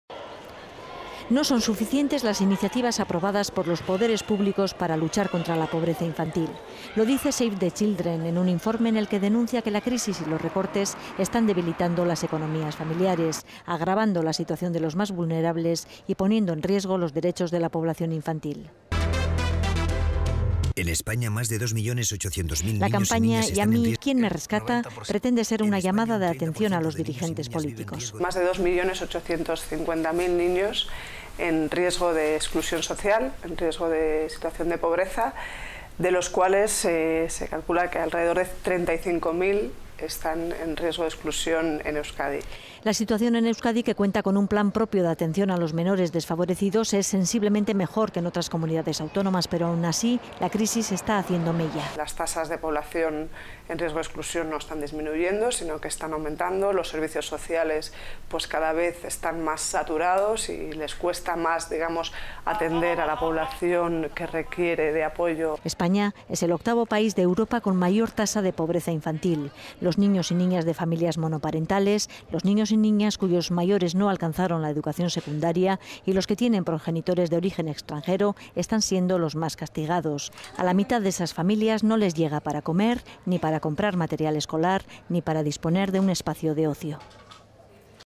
''Save the children'' denuncia el aumento de la pobreza infantil en España
El informe asegura que la crisis ha puesto en peligro los derechos de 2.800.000 niños y niñas. Se estima que en Euskadi son 35.000, los menores de 14 años, en situación de vulnerabilidad.